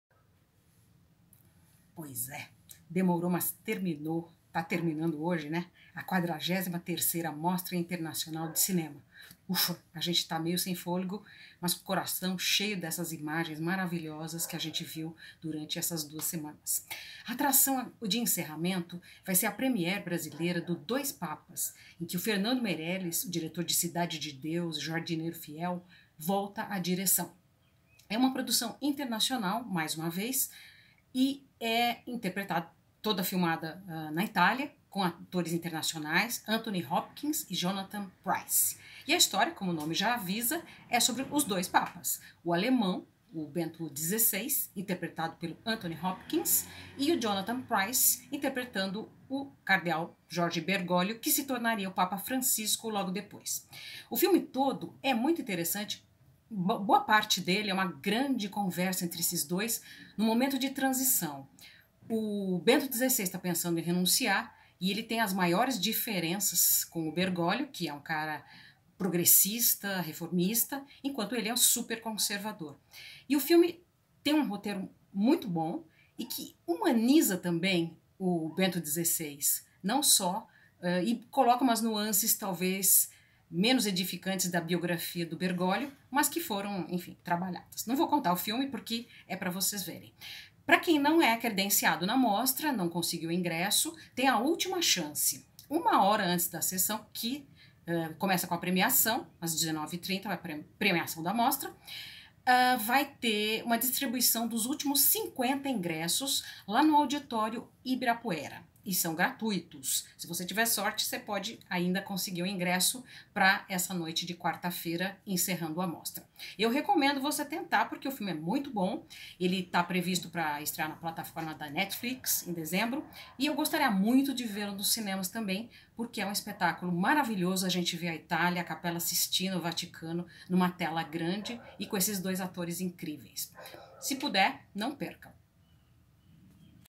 "Dois papas", de Fernando Meirelles, encerra a Mostra Internacional de Cinema
A partir das 19h30, nesta quarta (30/10), no auditório Ibirapuera, a cerimônia de premiação encerra a 43ª edição da Mostra, que resistiu bravamente às incertezas da política e economia apostando no vigor da arte.

Logo depois dos prêmios, a atração da noite é a première brasileira de "Dois Papas", o novo longa de Fernando Meirelles, que já passou no Festival de Toronto.

Até o final de semana, deverá ainda haver uma repescagem de alguns títulos, a ser divulgada.